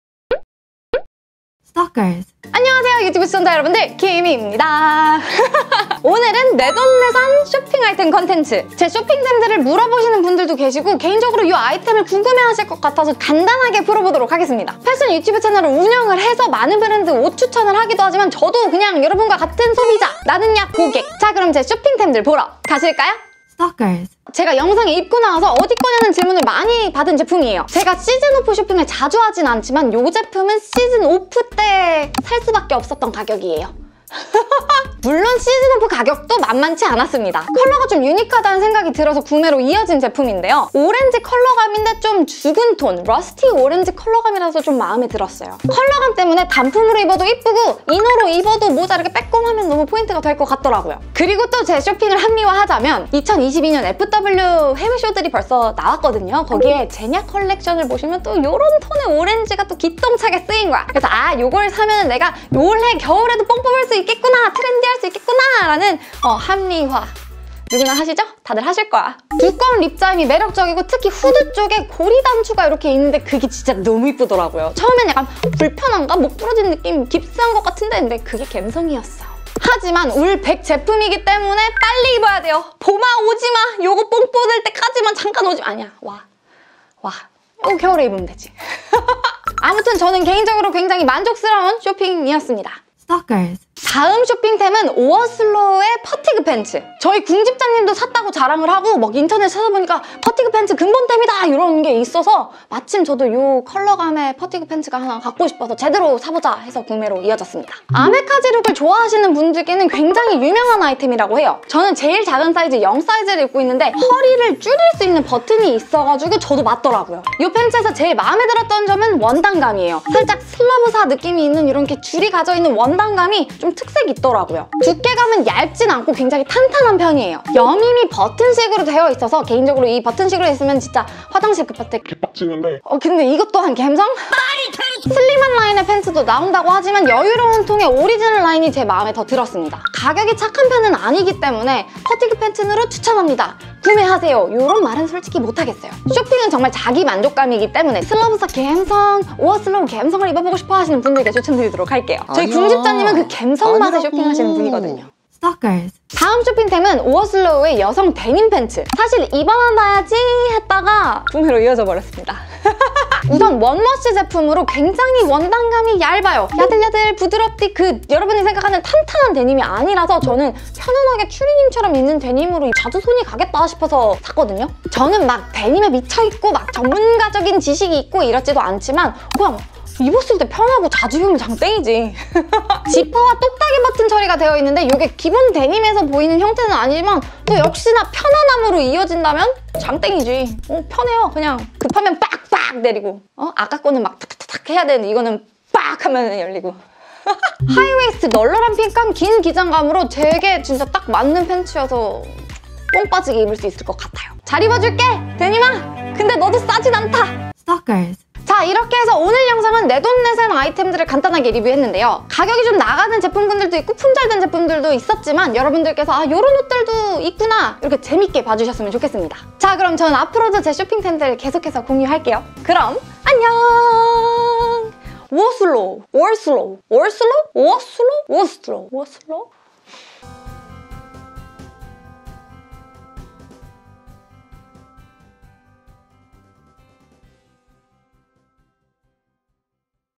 내 돈주고 샀으니 솔직하게 말할게요
오늘은 내돈내산 쇼핑 컨텐츠를 준비했습니다!
가격대가 있는 제품들이라 솔직하게 추천...드리기는 조금 힘들지만
그래도 확실히 비싼 값을 한다고 생각하기에
여러분께도 소개시켜드리면 좋을 것 같아서 준비했습니다ㅎㅎ

🌟PRODUCT INFO🌟 - 키미 스펙 167cm 50kg

(1) [오라리] SUPER FINE WOOL RIB KNIT BIG P/O PARKA ORANGE - 사이즈 3 착용

(2) [오어슬로우] 01 5002 US ARMY FATIGUE PANTS ORIGINAL FIT -사이즈 0 착용

(3) [오어슬로우] 00 1040 HIGH WAIST DENIM ‘JASMIN’ FOR WOMEN -사이즈 1 착용

0:00 오프닝
0:23 SUPER FINE WOOL RIB KNIT BIG P/O PARKA
1:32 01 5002 US ARMY FATIGUE PANTS ORIGINAL FIT
2:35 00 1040 HIGH WAIST DENIM ‘JASMIN’ FOR WOMEN
3:27 엔딩